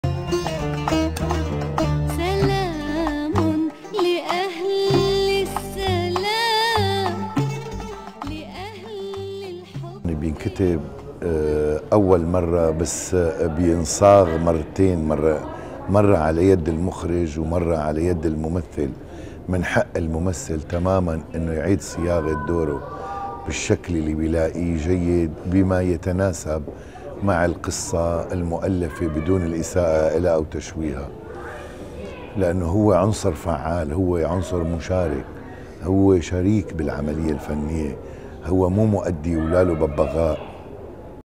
Wafik Al-Za3eem Interview Pt 6 ‎المخرج ‎وفيق الزعيم و ‎
‎‪Wafik Al-Za3eem, the famous syrian actor of "Bab Al Hara" here interviewed in Dearborn Michigan at the beautiful Hyatt Regency as he talks about THE PRODUCER ... Enjoy and don't forget to comment, share with friends, like and subscribe :) for more audio and video الله يرحمك December 17, 2009